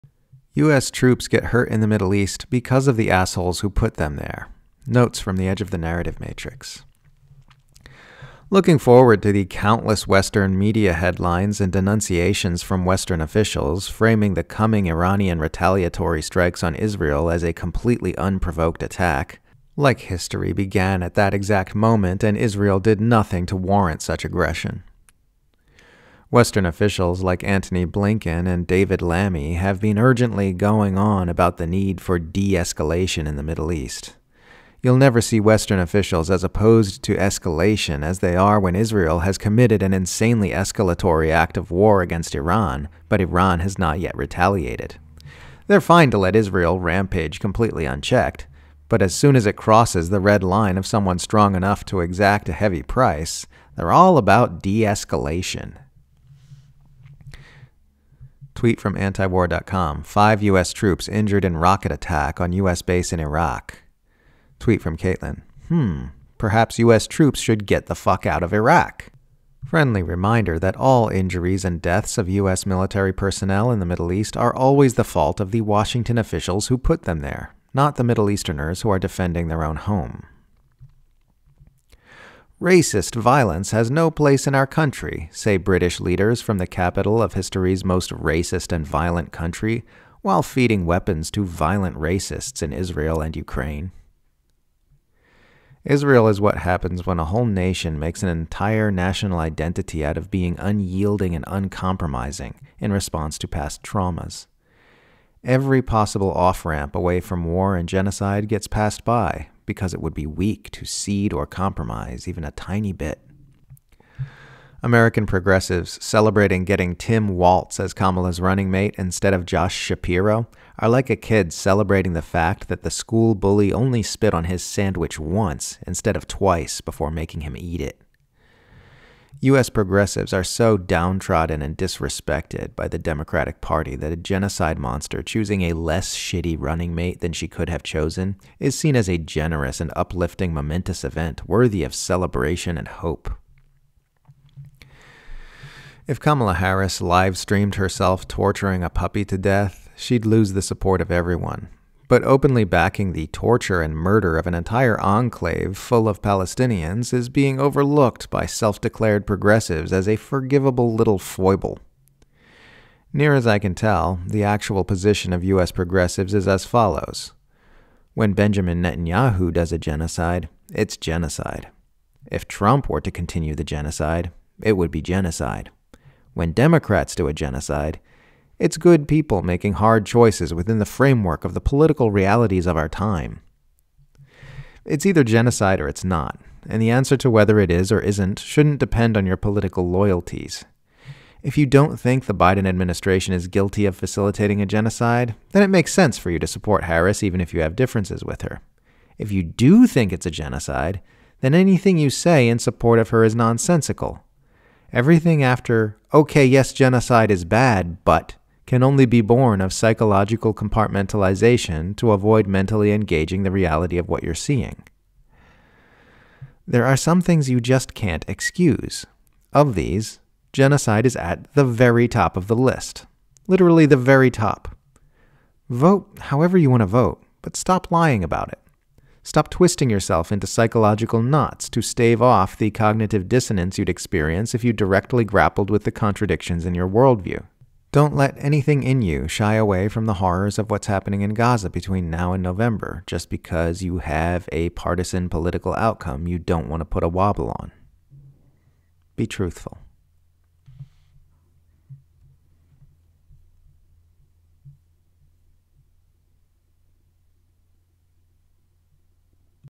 US Troops Get Hurt In The Middle East Because Of The Assholes Who Put Them There
Friendly reminder that all injuries and deaths of US military personnel in the middle east are always the fault of the Washington officials who put them there, not the middle easterners who are defending their own home.